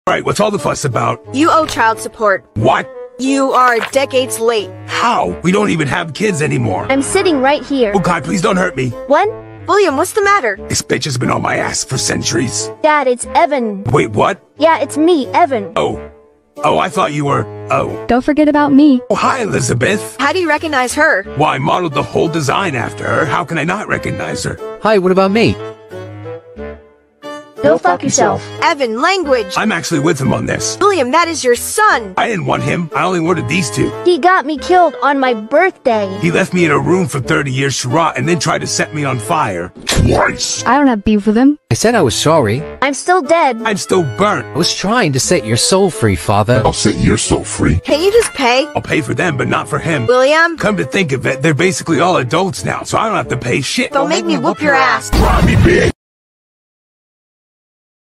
Springtrap owes child support (Flipaclip)
Omg why was this so hard to make help 😭

This originally took me 3 days to make but I had not 1 but 3 school projects thats I had to finish 😔
Anyways I finally finished it though :D

Also Its getting closer to Christmas..😦

Anyways :

Time - 3 days (Not counting the time I didn't work on it)
Things I used : Flipaclip and my finger

Hardest character to animate : Springtrap
Easiest character to animate (Besides Elizabeth and C.C) : Micheal

By the way, pretend Mrs. Afton has green eyes i was tired 😔

Anyways that all so have a nice day/night! 👾